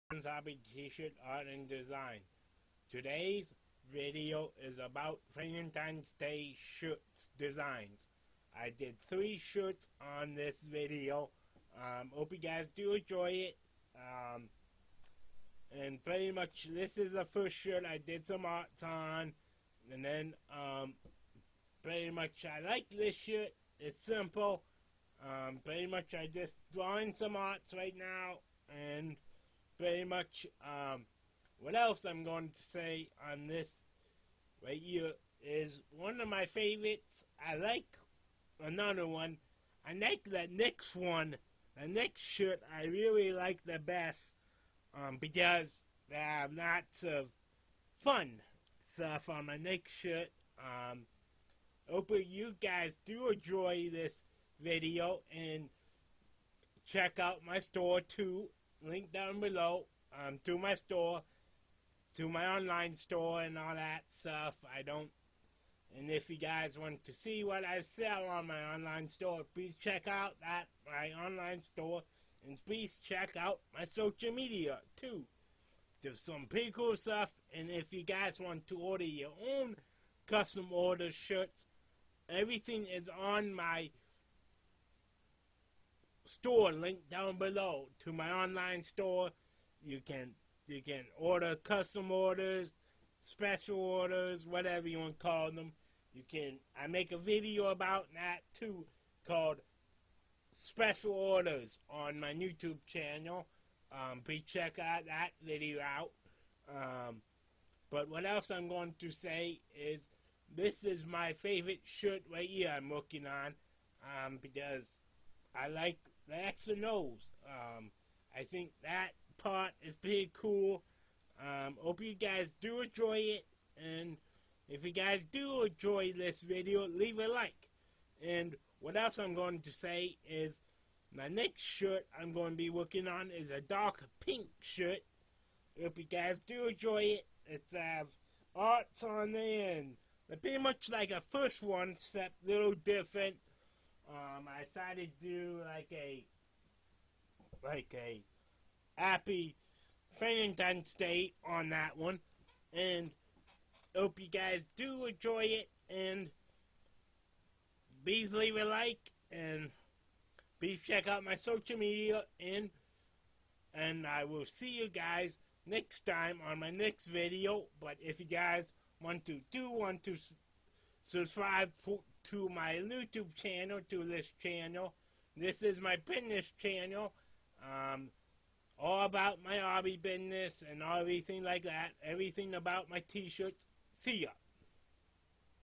fun valentine's day designs
I worked on three awesome shirt designs for valentine's day